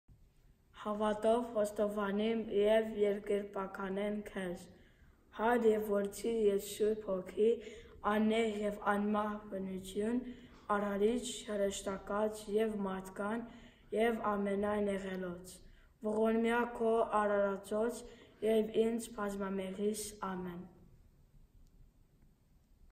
Հաւատով Խոստովանիմ Ա․ աղօթք - Հայերէն
Prayers from In Faith I Confess - St Nerses Shnorhali read by various young parishioners throughout the Prelacy parishes.